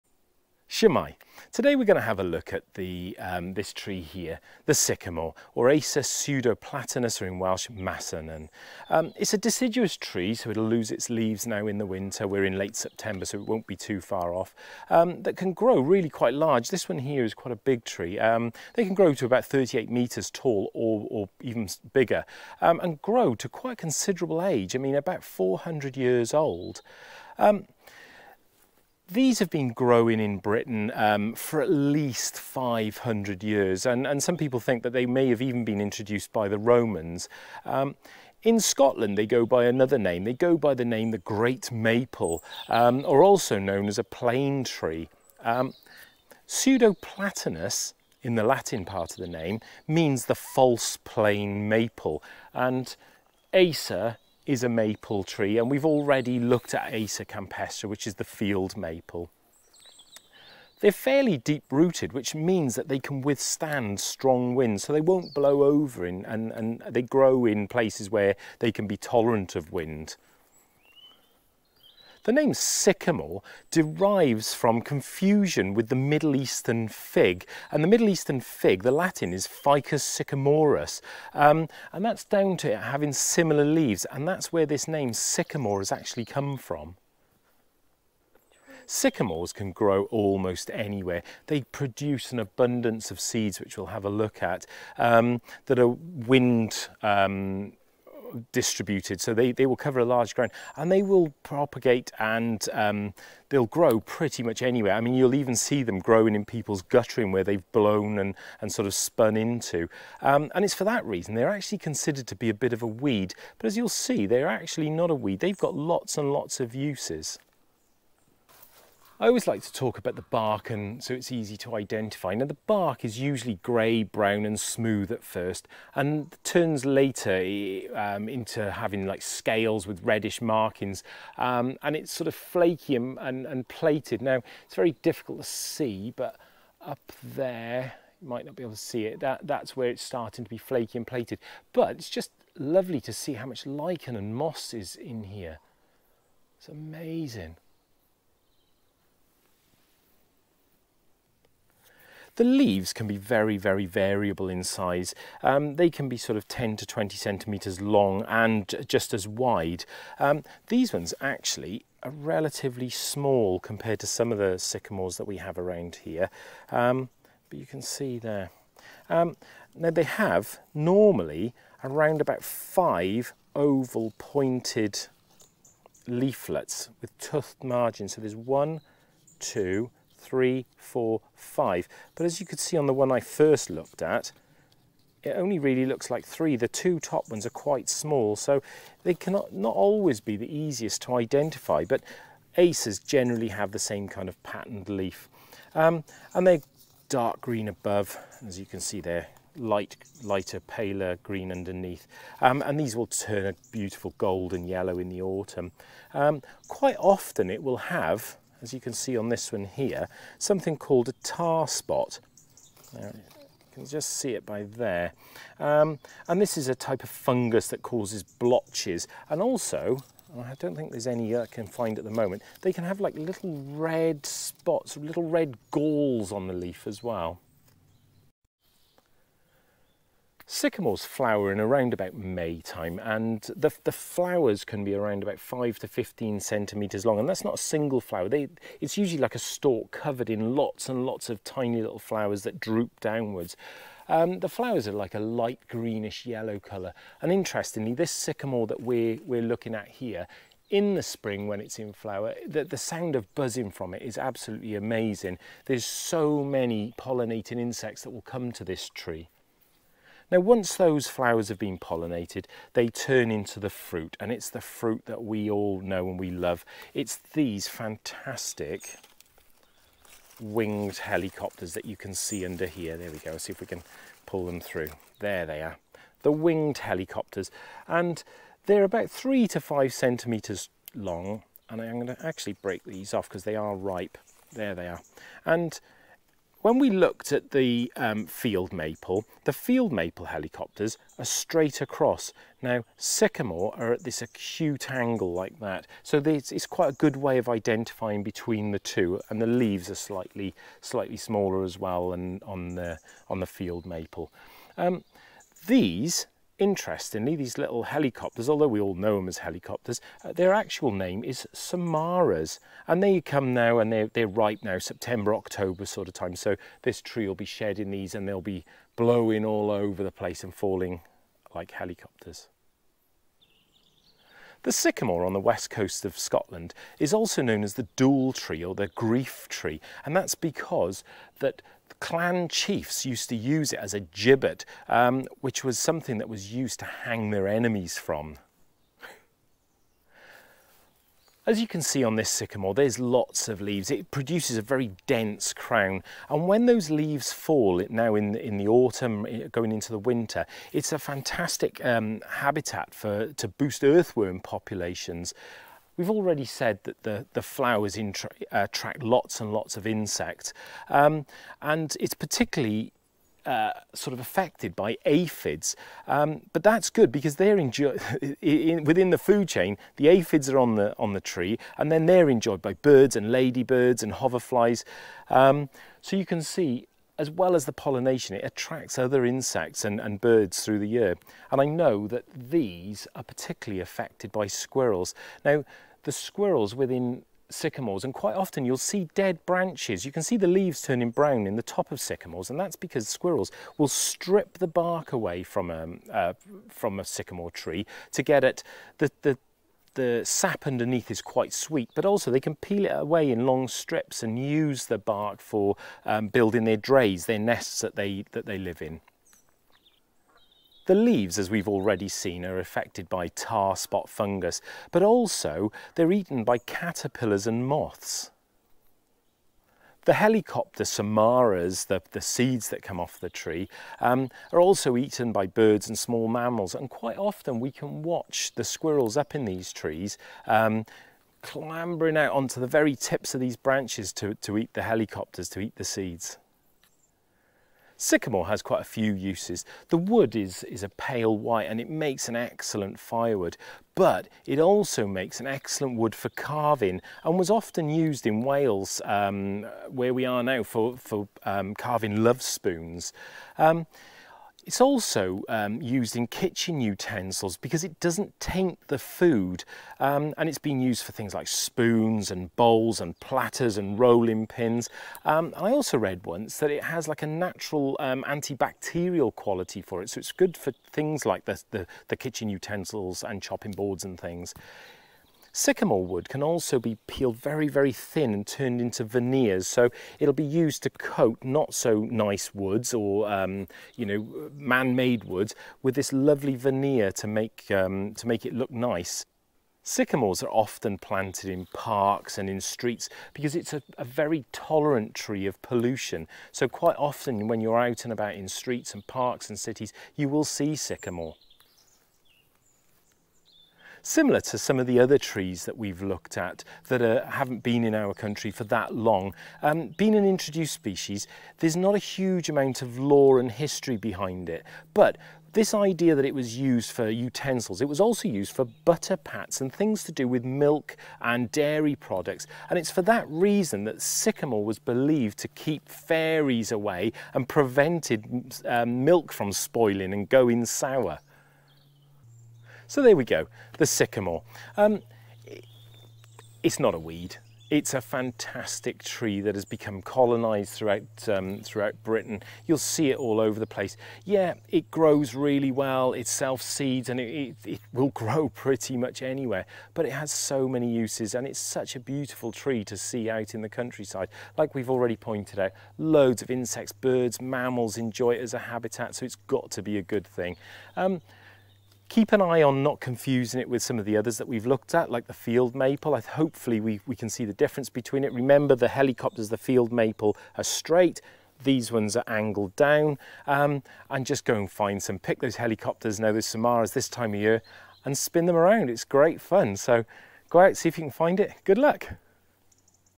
Sycamore
Sycamore: nuisance, weed, "Grief Tree", or a tough and tenacious survivor?  Find out what samaras are, and let Mr.C reveal the Sycamore's real beauty.